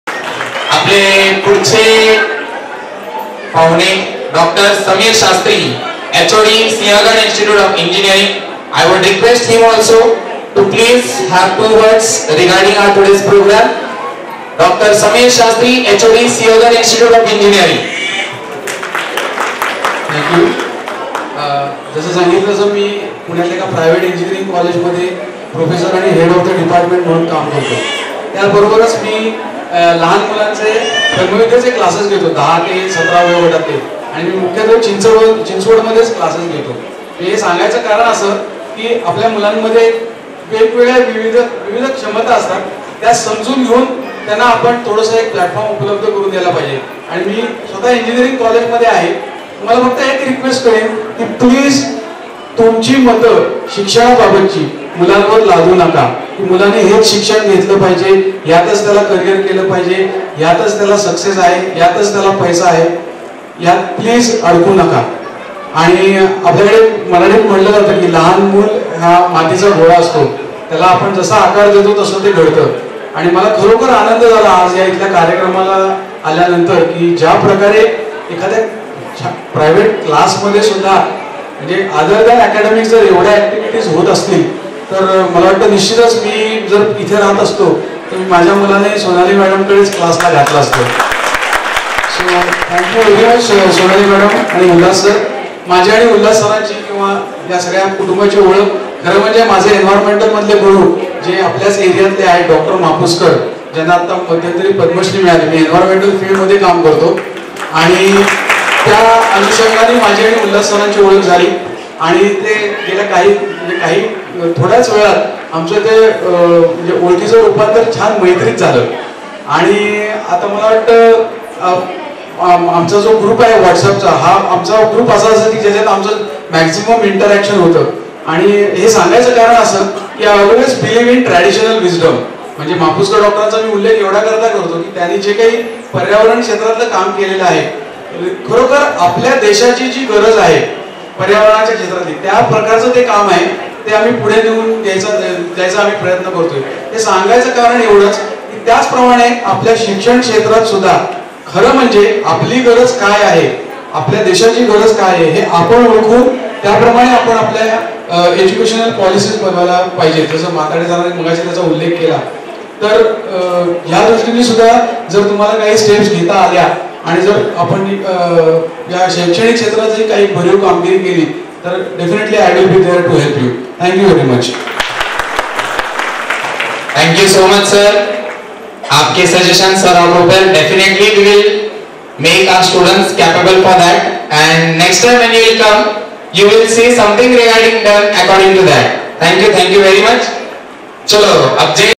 Sonalis Academy Annual Function 2016-17 Dr Sameer Shastri Speech
Sonalis Academy, Mrs. Sonali Ulhas Mapuskar, Annual Function 2016-17 Part I, Mrs. Harsha Prashant Joshi was Chief Guest to the Function along with Smt. Chhaya Habbu and Mrs. Sumita Bhattacharya as Guest of Honour.